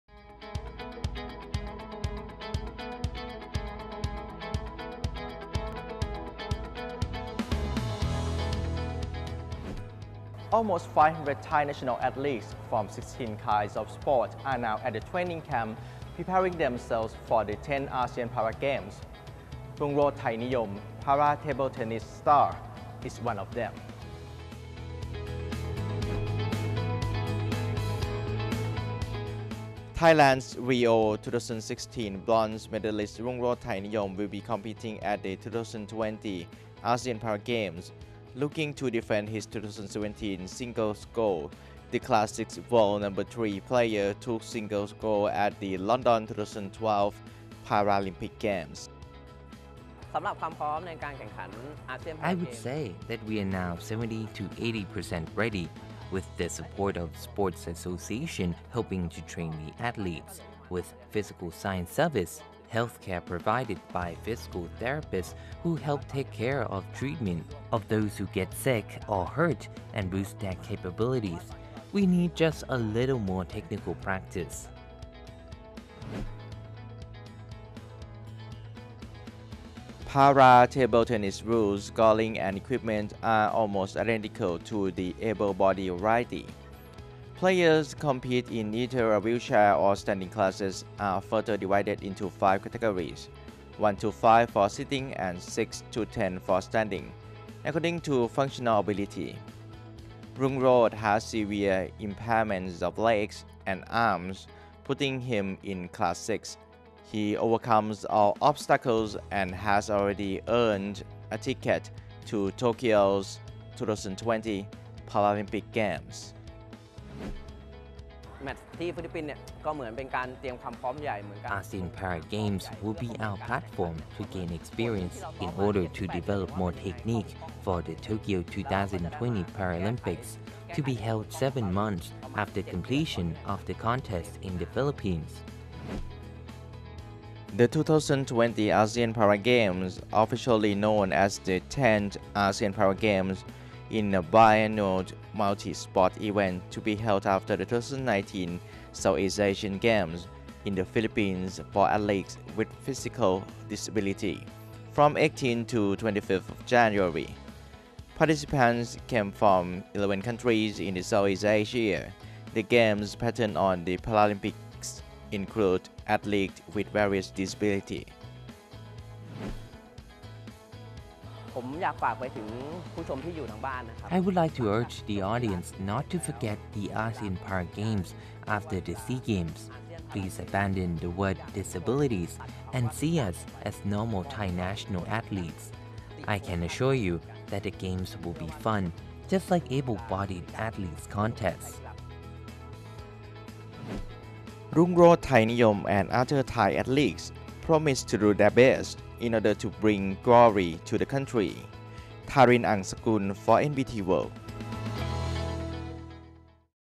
London gold medallist heads Thai Challenge in the Philippines
Led by table-tennis star Rungroj Thainiyom, who won gold at the London 2012 Paralympic Games, Thailand leads the challenge for honors at the forthcoming 2020 ASEAN Para Games.